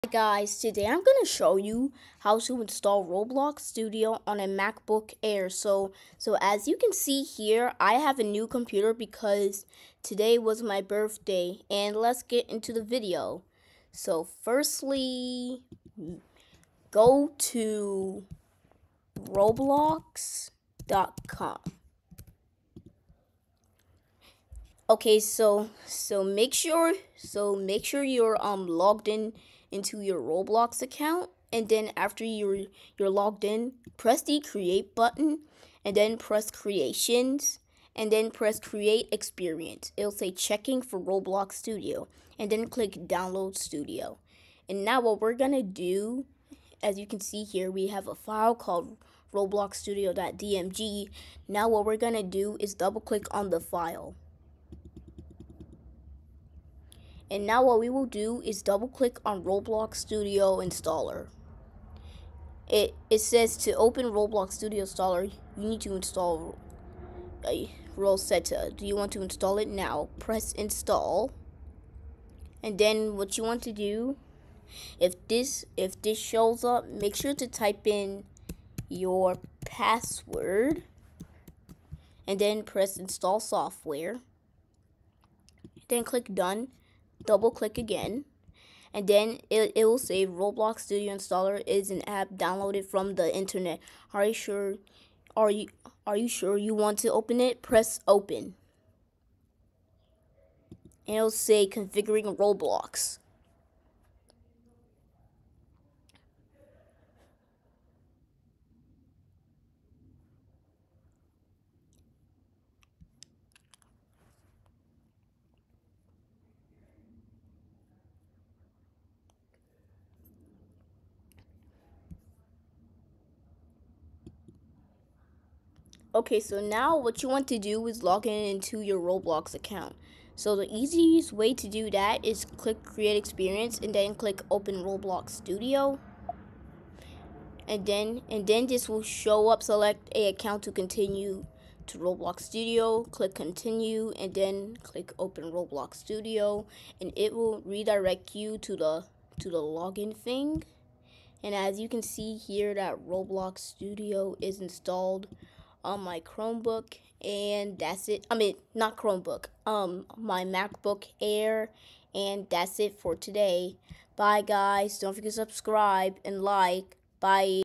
How to install Roblox Studio on a MacBook Air.
Hi guys today i will show you how to install Roblox Studio on a Macbook Air.